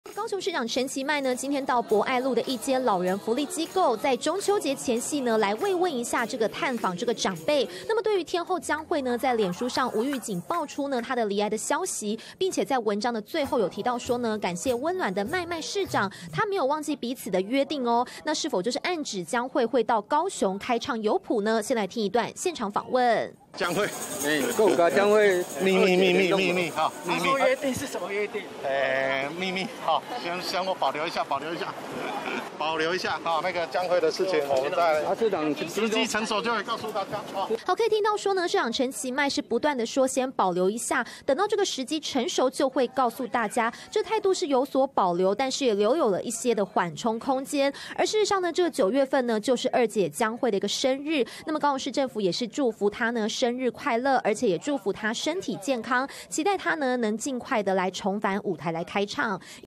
天后江蕙爆罹癌睽違9年將開唱 陳其邁最新回應－民視新聞
【民視即時新聞】台語歌后江蕙日前宣布睽違9年復出開唱，將在國慶晚會演出，先前高雄市長陳其邁，也積極爭取江蕙到高雄開唱，今天陳其邁到老人福利機構，中秋節前慰問長輩，針對江蕙是否到高雄開唱，陳其邁也做出最新回應，現場情形，連線給記者劉尹淳，來告訴我們。